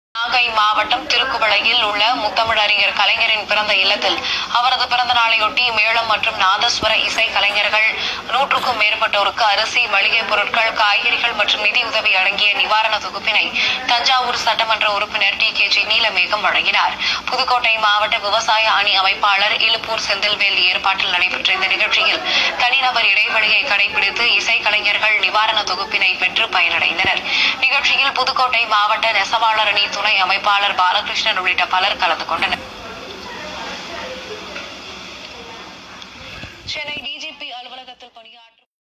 முத்தமிழ் அறிஞர் கலைஞர் அவர்கள் 97வது பிறந்த நாள் நிகழ்வின் செய்திக்குறிப்பு.
முத்தமிழ் அறிஞர் கலைஞர் அவர்கள் 97வது பிறந்த நாளை முன்னிட்டு திருக்குவளையில் உள்ள அவரது இல்லத்தில் தவில் மற்றும் நாதஸ்வரக் கலைஞர்களுக்கு நிவாரணம் வழங்கிய நிகழ்வின் செய்திக்குறிப்பு...